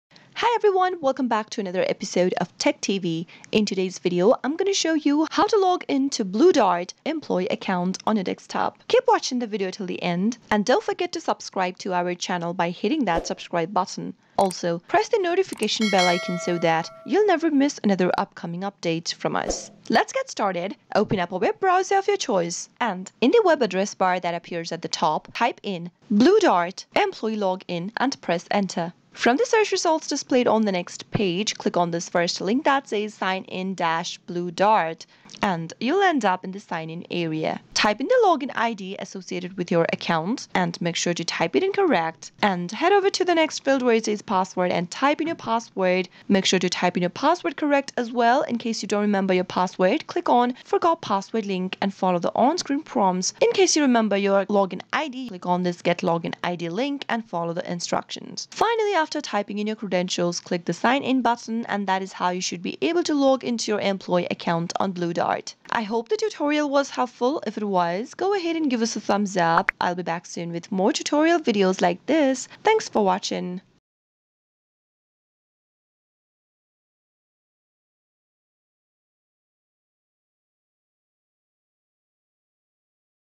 How to Login to Blue Dart Employee Account | Blue Dart Employee Sign In
In this video I am going to guide you How to Login to Blue Dart Employee Account.

CHAPTERS
0:00 - Intro
0:19 - How to Login to Blue Dart Employee Account
1:05 - Outro

Hey EveryOne!